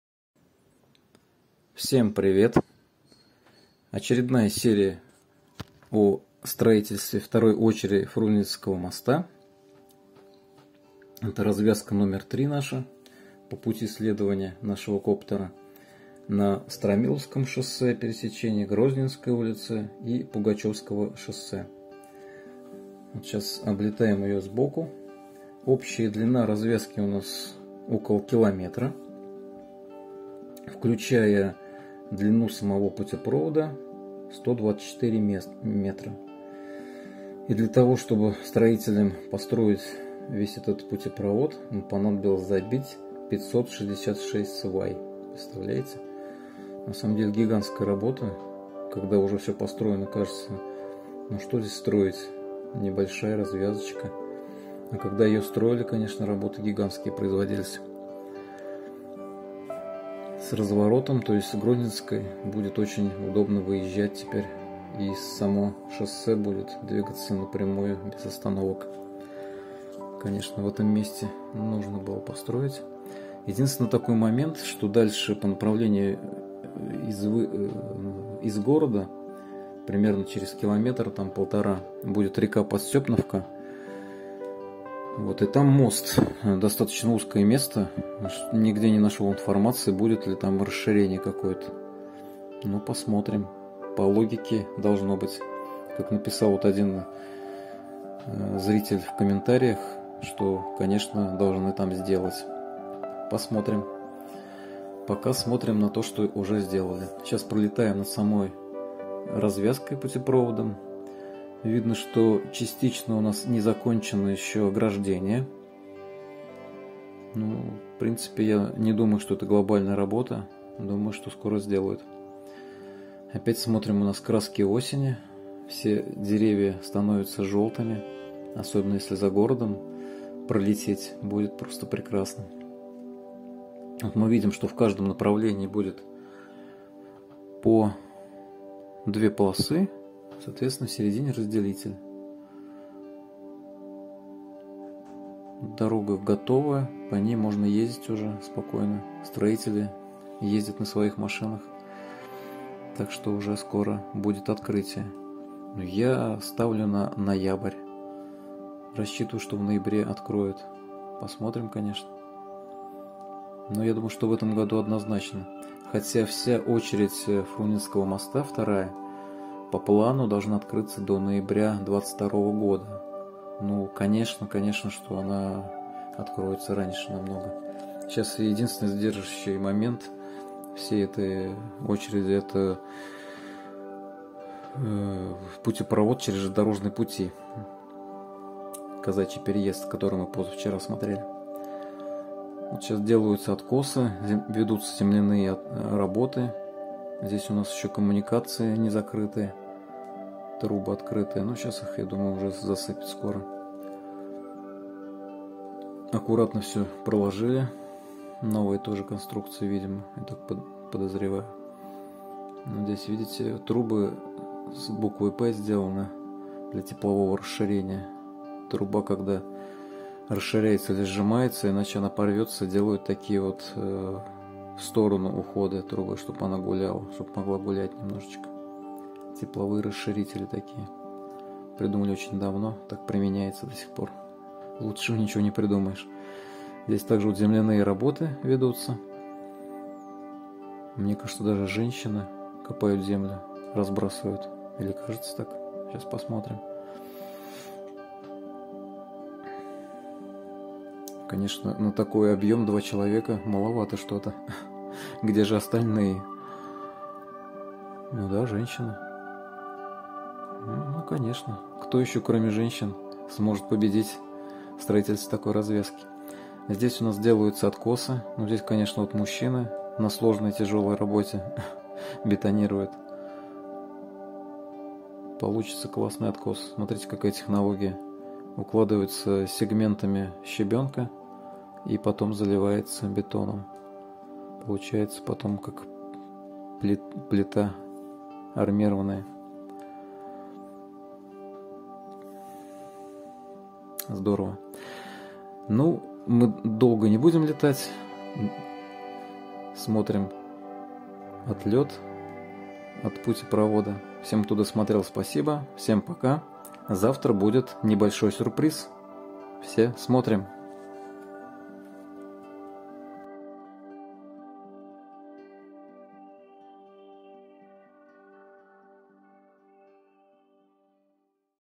СТРОИТЕЛЬСТВО РАЗВЯЗКИ НА ПУГАЧЁВСКОМ ТРАКТЕ - ГРОЗНЕНСКОЙ УЛ./САМАРА/РОССИЯ/ОКТЯБРЬ 2021
Завершается строительство грандиозной развязки! Скоро поедем по ней!